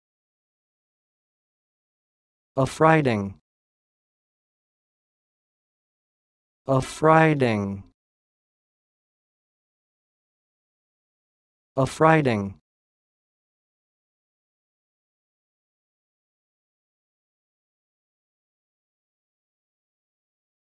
Learn how to say this word: "Affrighting"
This is the definition of "Affrighting":
p. pr. & vb. n.  of Affright